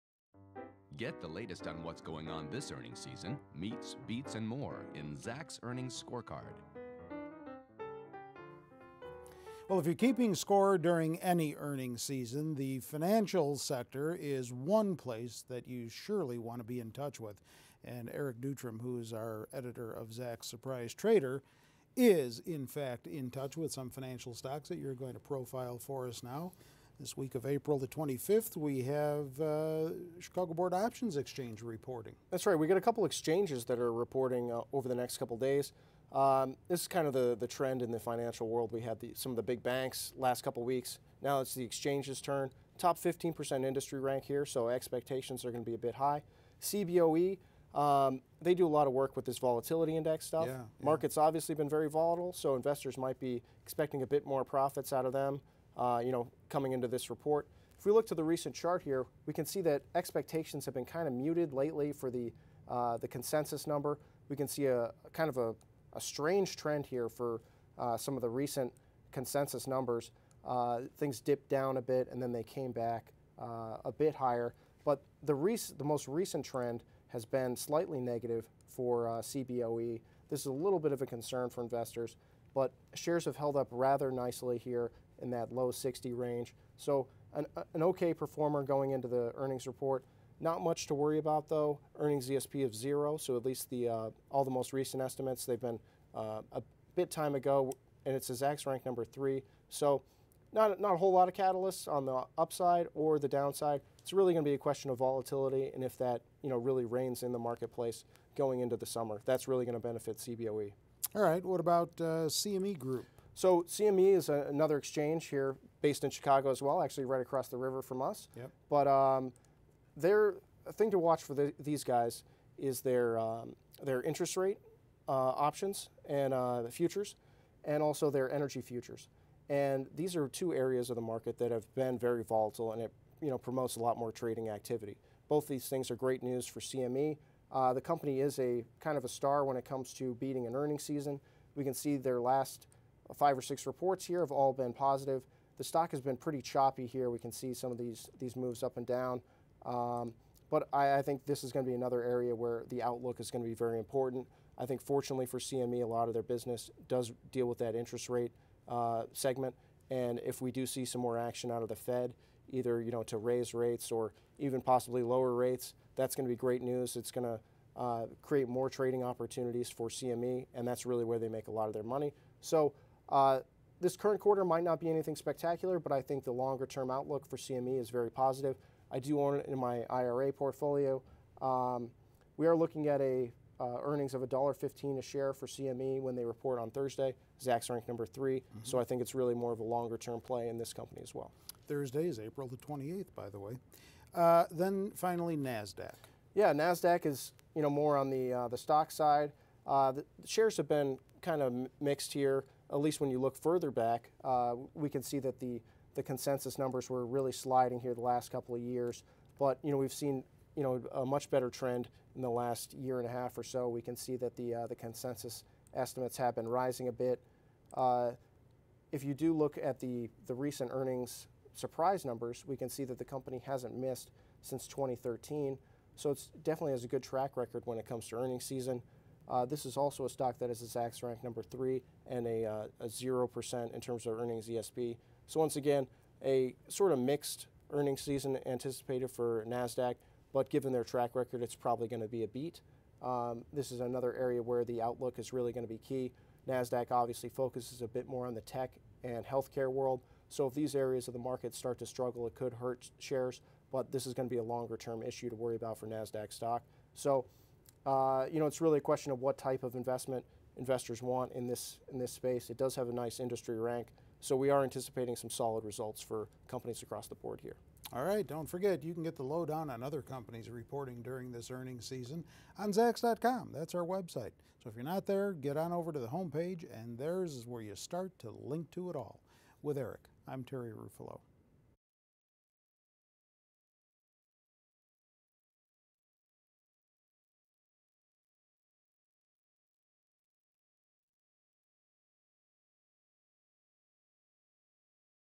Financial Stocks: 3 to Watch for Earnings (CME, CBOE, NDAQ)
With big bank stocks out of the way, the exchanges are up next for earnings in the financial sector. And thanks to some recent volatility, expectations are a bit higher but can these companies deliver? We take a look in this short earnings preview video.